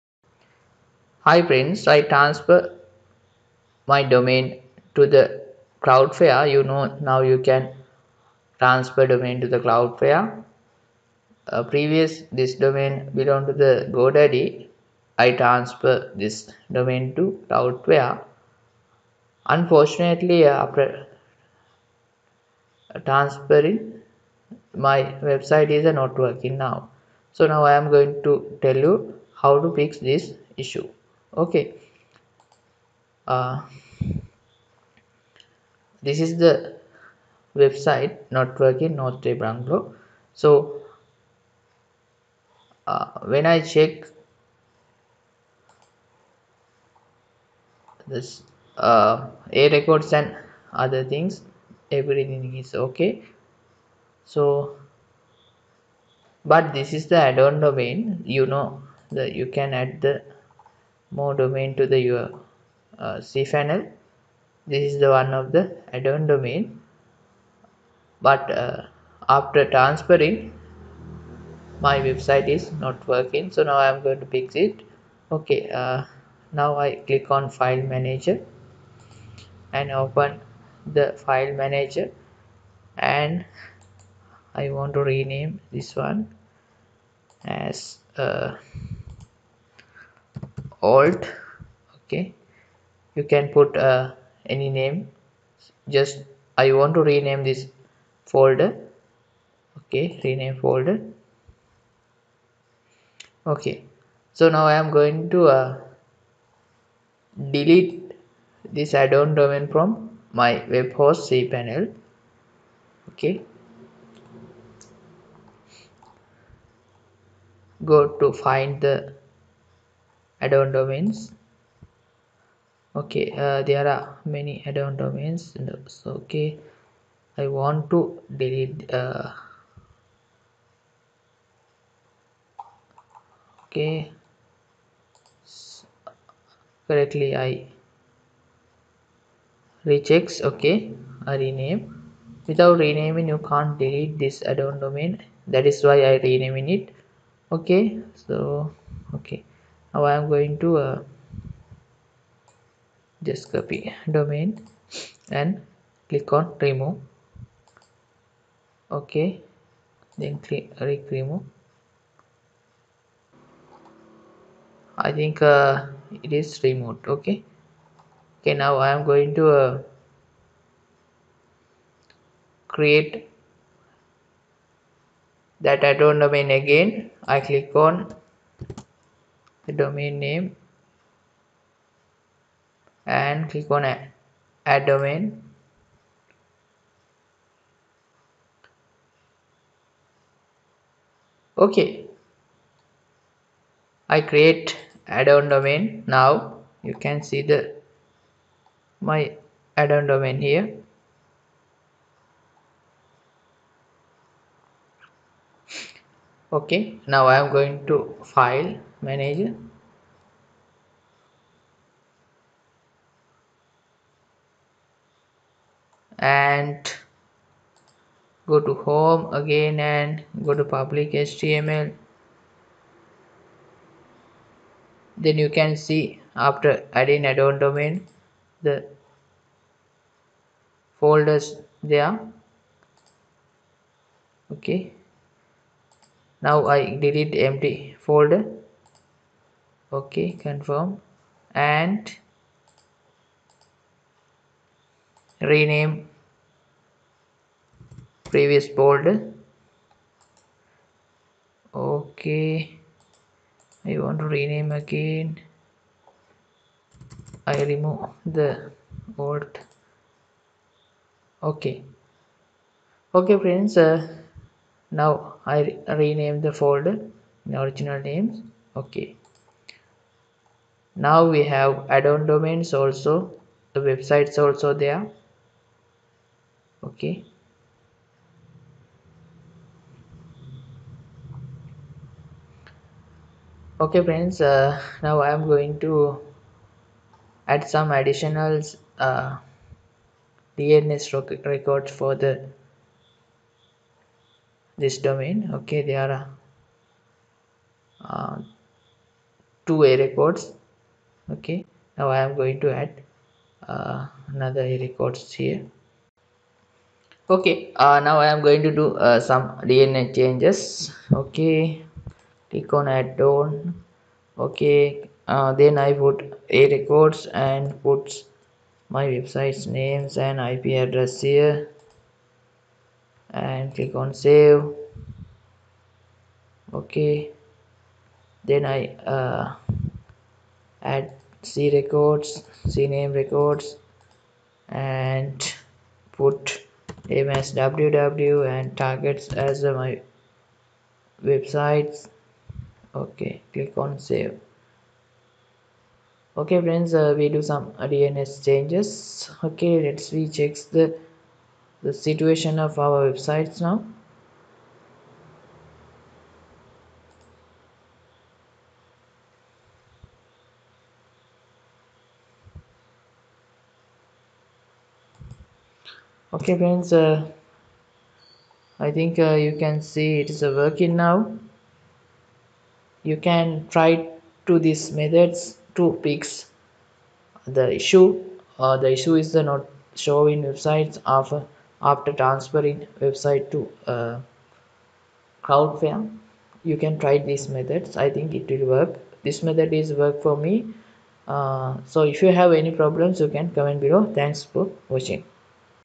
How to fix website not working after domain transfer to cloudflare
I transferred a domain to Cloudflare. the transformation was successful but the website is not working after transferring  this video shows  How to fix the website not working after domain transfer to Cloudflare